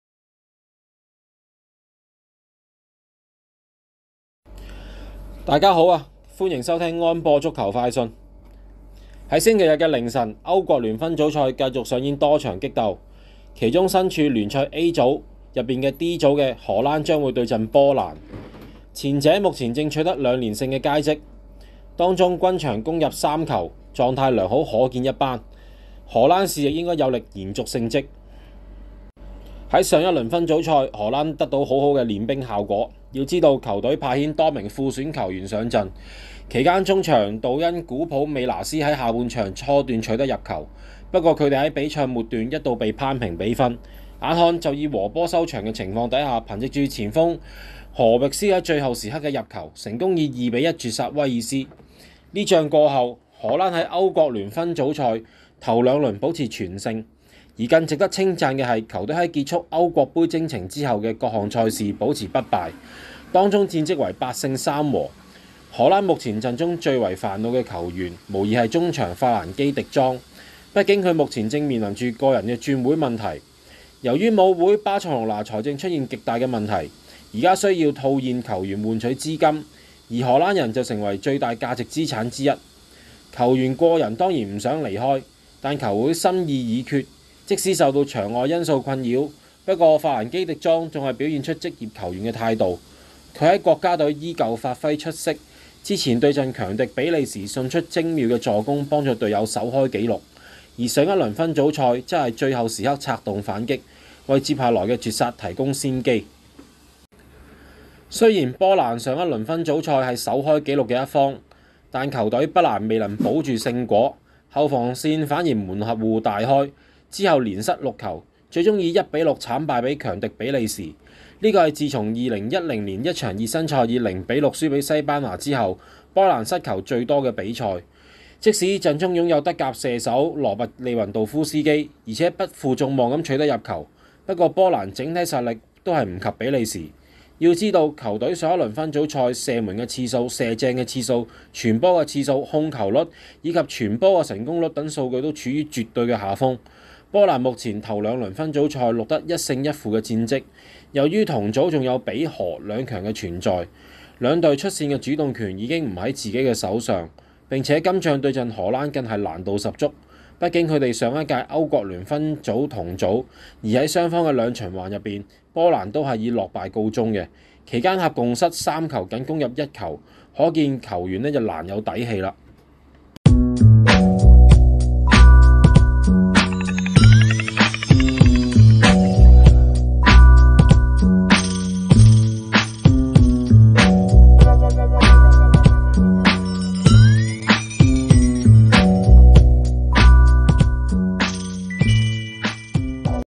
【歐國聯-賽前分析】2022-06-12 荷蘭 VS 波蘭 | 荷蘭贏「波」機率大 [聲音旁述: TooBy]
荷蘭 贏「波」機率大
週日凌晨，歐國聯 分組賽繼續上演多場激鬥，其中身處聯賽A組D組的荷蘭將對陣 波蘭 ，前者目前正取得兩連勝的佳績，當中均場攻入3球，狀態良好可見一斑，荷蘭是役有力延續勝績。

上輪分組賽，荷蘭得到很好的練兵效果，要知道，球隊派遣多名副選球員上陣，期間中場杜恩.古普美拿斯在下半場初段取得入球，不過他們在比賽末段一度被扳平比分，眼看就要以和局收場的情況下，憑藉前鋒禾特.域賀斯在最後時刻的入球，成功以2比1絕殺威爾斯。此役過後，荷蘭在歐國聯分組賽頭2輪保持全勝，而更值得稱讚的是，球隊在結束歐國盃征程之後的各項賽事保持不敗，當中戰績為8勝3和。荷蘭目前陣中最為「煩惱」的球員無疑是中場法蘭基.迪莊，畢竟他目前正面臨著個人轉會問題，由於母會巴塞隆拿財政出現極大的問題，現在需要套現球員換取資金，而荷蘭人就成為最大價值資產之一，球員個人當然不想離開，但球會心意已決。即便受到場外因素困擾，不過法蘭基.迪莊還是表現出職業球員的態度，他在國家隊依舊發揮出色，此前對陣強敵比利時送出精妙助攻幫助隊友首開紀錄，而上輪分組賽則是在最後時刻策動反擊，為接下來的絕殺提供先機。

雖然波蘭上輪分組賽是首開紀錄的一方，但球隊不但未能保住勝果，後防線反而門戶大開，他們之後連失6球，最終以1比6慘敗給強敵比利時，這是自從2010年在1場熱身賽以0比6負於西班牙之後，波蘭失球最多的比賽。縱使陣中擁有德甲射手王羅拔.利雲度夫斯基，而且亦不負眾望地取得入球，不過波蘭整體實力還是不及比利時，要知道，球隊上輪分組賽的射門次數、射正次數、傳球次數、控球率以及傳球成功率等數據都處於絕對下風。波蘭目前頭2輪分組賽錄得1勝1負的戰績，由於同組還有比、荷兩強的存在，球隊出線主動權已經不在自己的手上，並且今仗對陣荷蘭更是難度十足，畢竟他們上屆歐國聯分在同組，而在雙方的兩循環中，波蘭都以落敗告終，期間合共失3球僅攻入1球，可見球員難有底氣。